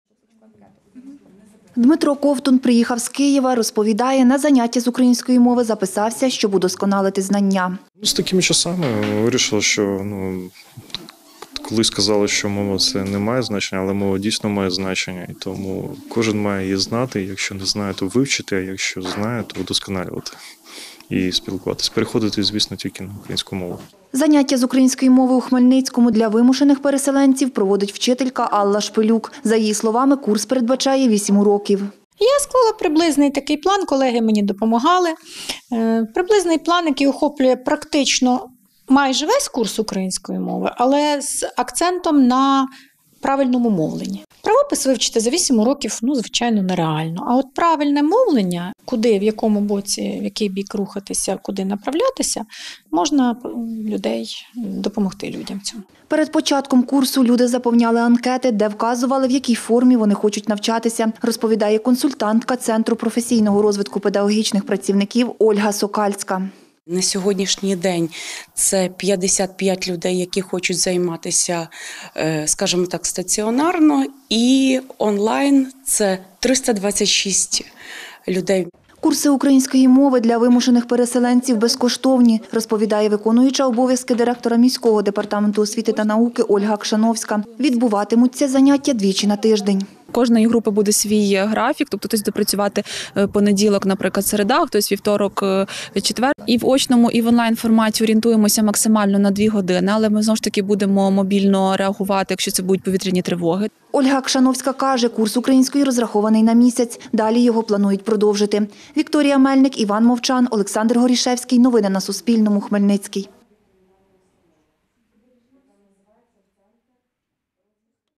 У Хмельницькому запрацювали курси української мови для вимушених переселенців
Курси української мови для вимушених переселенців запрацювали в обласному центрі. Про це розповіла виконуюча обов’язки директора департаменту освіти та науки Хмельницької міської ради Ольга Кшановська. За її словами, організували курси, бо був на них попит від гостей міста. Заняття української проходитимуть як в очно, так і дистанційно. Детальніше дивіться далі.